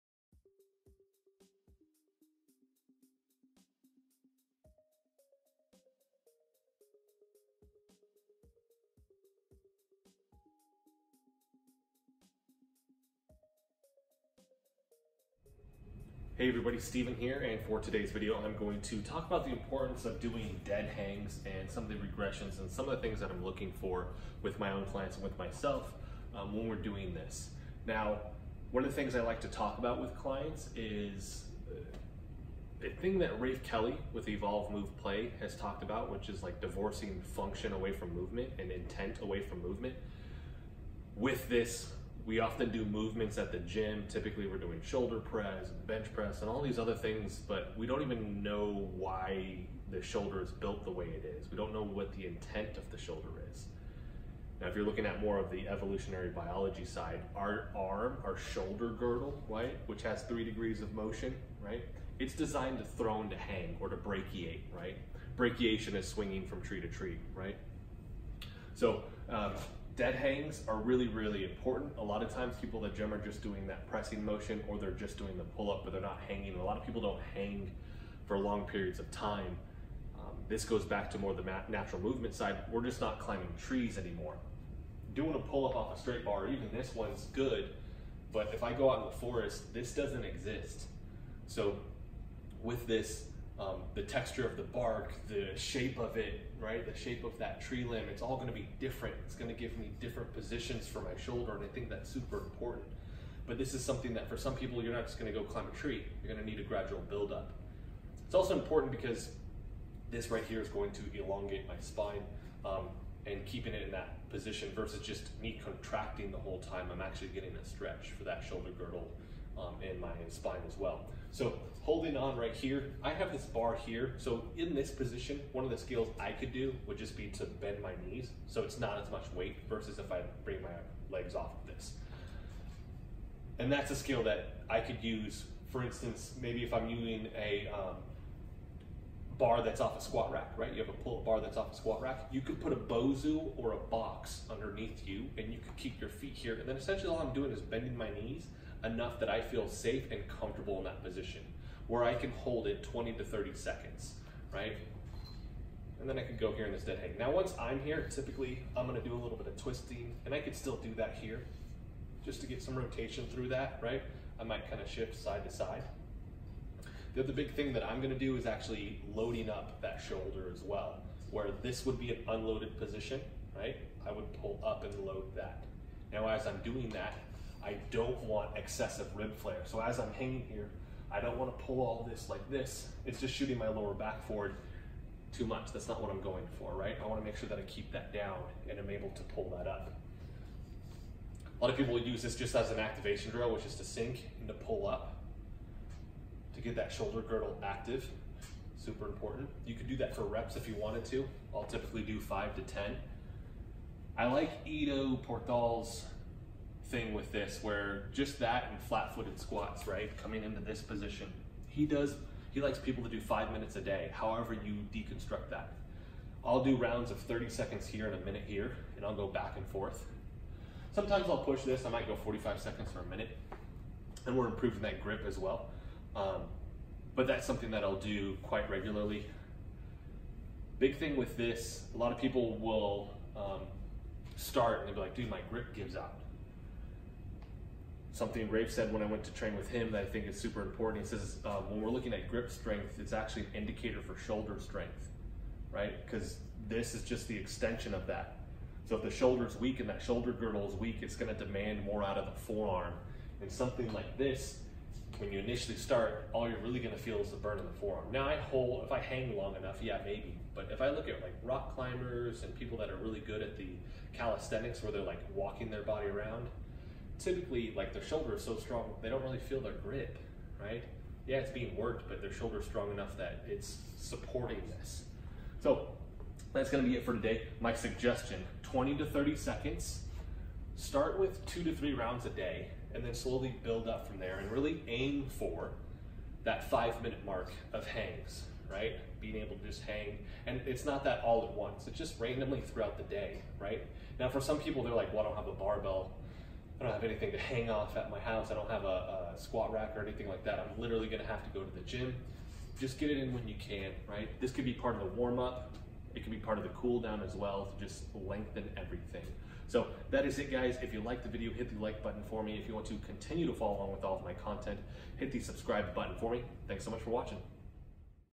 Why DeadHangs Are Important & How to Scale Them
Dead hangs are important for shoulder health and are often over looked in peoples fitness programs.  I do these for 30 seconds 3-5 rounds a day typically and show you how to scale this movement in the video.